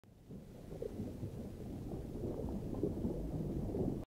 pike attack in fishtank2
Rendered in Lightwave and post processed in virtual dub